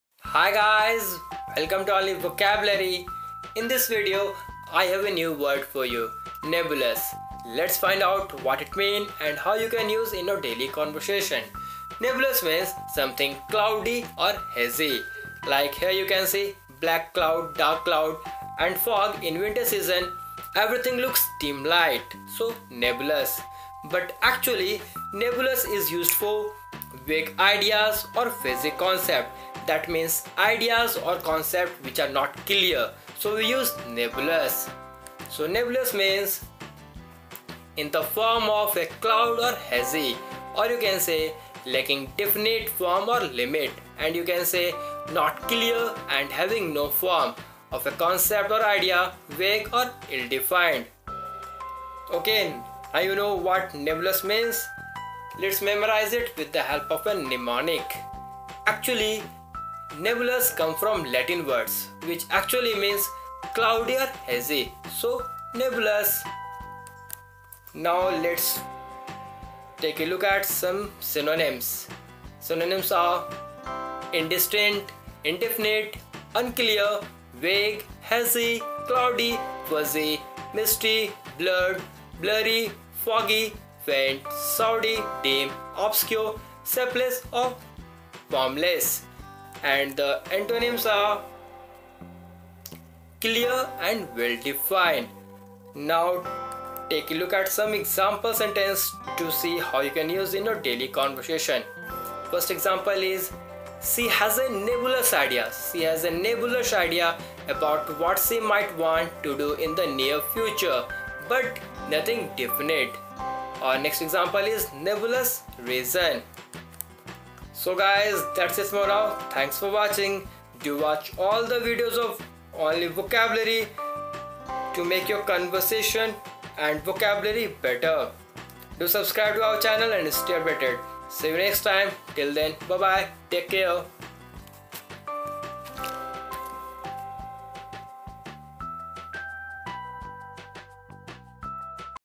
(English) Nebulous meaning #4 {GRE, SAT, CAT, GMAT, CSAT, UPSC, SSC, Banking}
"Only vocabulary " is a video Series in which you learn new words daily with pictures, synonyms,  antonyms, unique remember tips and illustrations.

Only Vocabulary
The daily Vocabulary

Nebulous
In the form of a cloud; hazy.
Lacking definite form or limits.
Not clear and having no form (of a concept, ideas) vague or ill-defined.

Synonyms
Indistinct, indefinite, unclear, vague, hazy, cloudy, fuzzy, misty, blurred, blurry, foggy, faint, shadowy, dim, obscure, shapeless, formless.

Antonyms
Clear,  well defined.

Example 1
She has a few nebulous ideas about what she might want to do in the future, but nothing definite.

Example 2
Nebulous reasons.